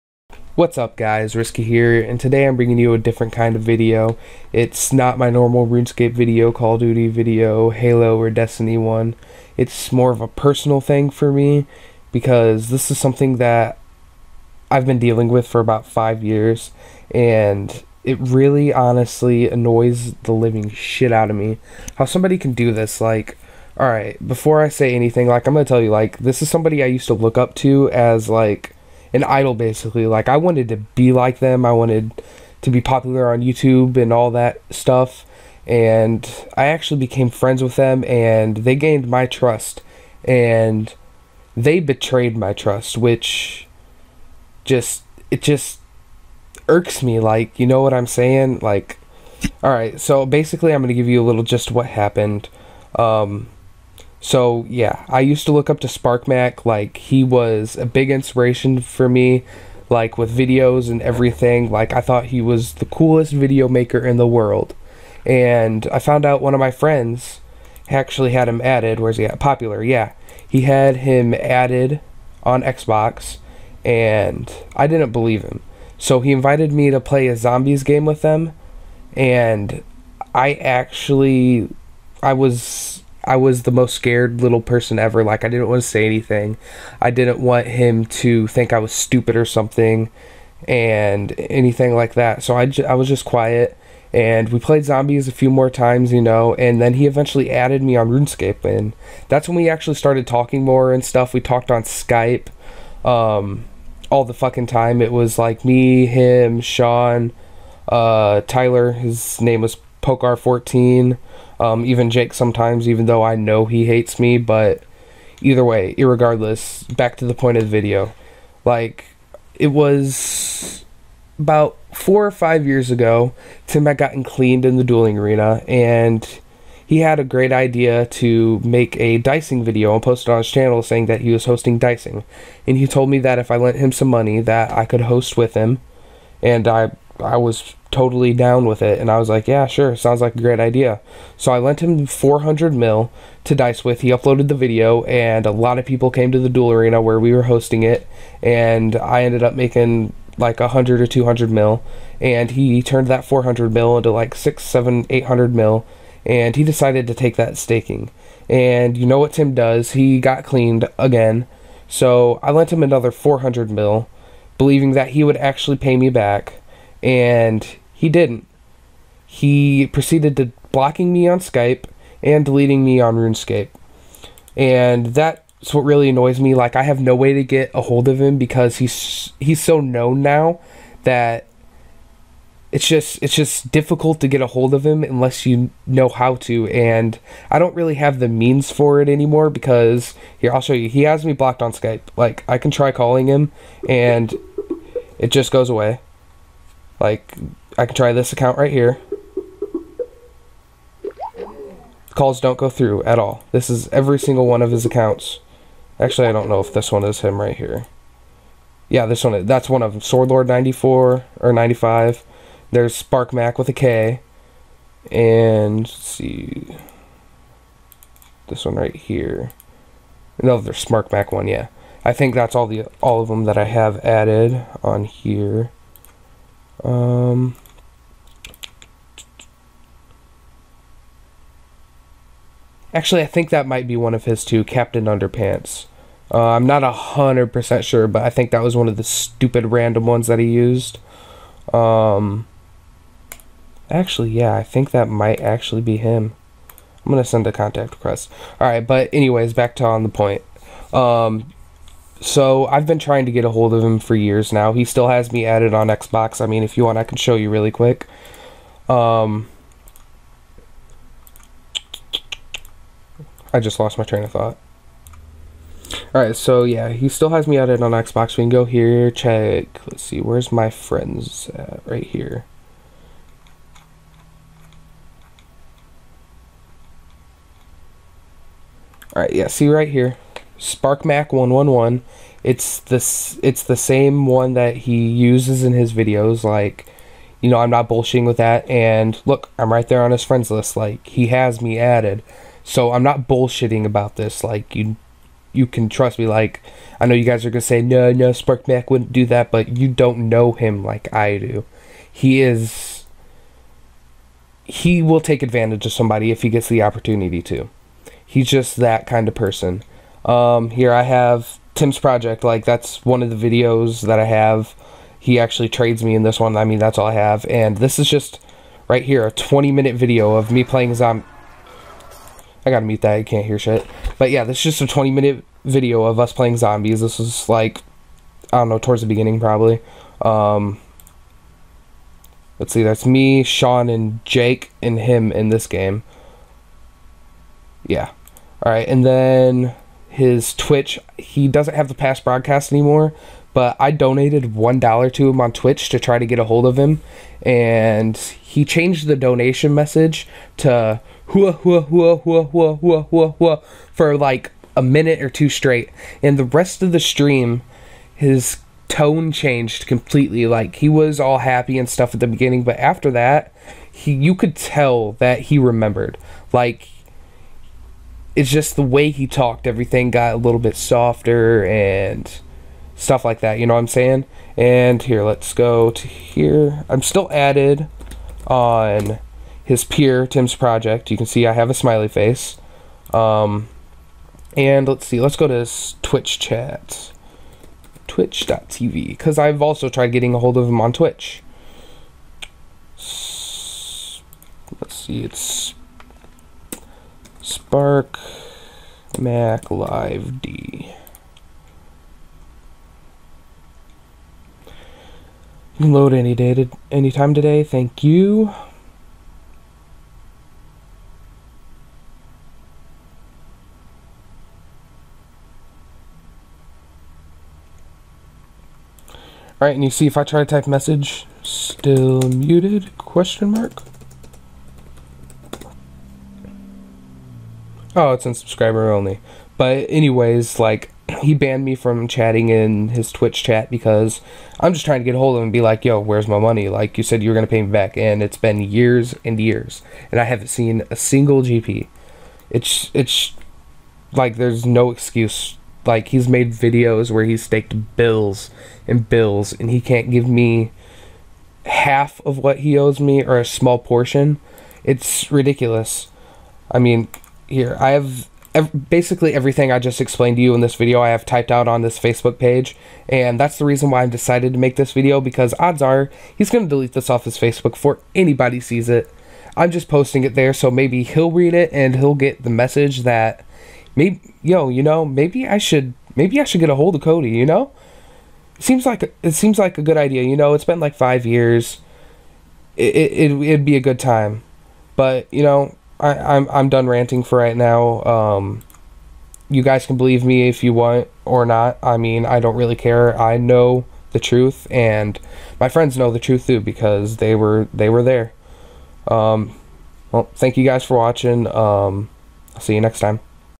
My experience with "Sparc Mac" the Scammer.
Everything I say in this video is %100 truth, and if you doubt me, just leave now. I am just trying to get a message across to you Tim, this is the result of ignoring me for 5 years.